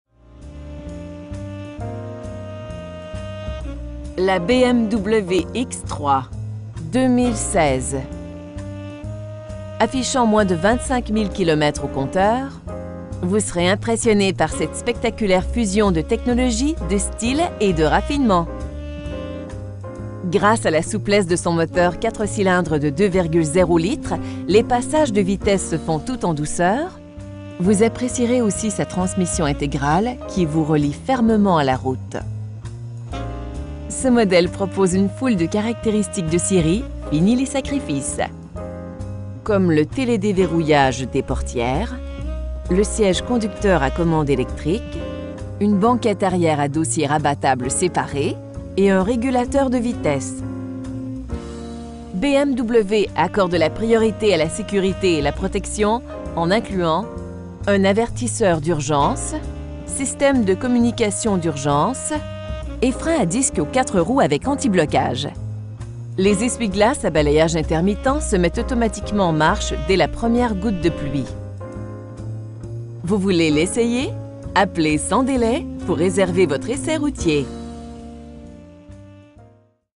2016 BMW X3 xDrive28Diesel CUIR TOIT NAV 257$/2SEM+TX
Centre Automobile Du Qu��bec
1700 Autoroute 440

��motions garanties avec la BMW��X3 2016. Affichant moins de 25 000 km au compteur, vous serez impressionn�� par cette spectaculaire fusion de technologie, de style et de raffinement. Gr��ce �� la souplesse de son moteur 4 cylindres de 2,0 litres, les passages de vitesses se font tout en douceur, vous appr��cierez aussi sa transmission int��grale qui vous relie fermement �� la route. Sa suspension bien calibr��e et coupl��e �� une syst��me de contr��le de la stabilit�� procure un superbe agr��ment de conduite, tant en conduite dynamique que tranquille. Ce mod��le propose une foule de caract��ristiques de s��rie, fini les sacrifices. Comme le t��l��d��verrouillage des porti��res, l'ouverture automatique de la glace du conducteur, un compte-tours, le si��ge conducteur �� commande ��lectrique, le r��troviseur central avec fonction anti��blouissement automatique, un afficheur de la temp��rature ext��rieure, une banquette arri��re �� dossiers rabattables s��par��s, et un r��gulateur de vitesse. Les garnissages de si��ges de cuir de haute qualit�� ajoutent au confort, un luxe toujours appr��ci�� du conducteur et des passagers. Avec des phares �� haute intensit�� lumineuse pour ��clairer la route, vous b��n��ficierez d'une visibilit�� optimale en toutes circonstances. Le syst��me de navigation int��gr�� procure des instructions �� chaque intersection, et les commandes de cha��ne st��r��o au volant permettent d'ais��ment r��gler le syst��me tout en conduisant. BMW accorde la priorit�� �� la s��curit�� et la protection en incluant coussins gonflables �� l'avant, rideaux gonflables pour prot��ger la t��te, syst��me antipatinage, syst��me d'assistance au freinage, appuis-t��te avant anti-coup de fouet, un avertisseur d'urgence, syst��me de communication d'urgence, et freins �� disque aux 4 roues avec antiblocage. Les essuie-glaces �� balayage intermittent se mettent automatiquement en marche d��s la premi��re goutte de pluie. Vous voulez l'essayer? Appelez sans d��lai pour r��server votre essai routier.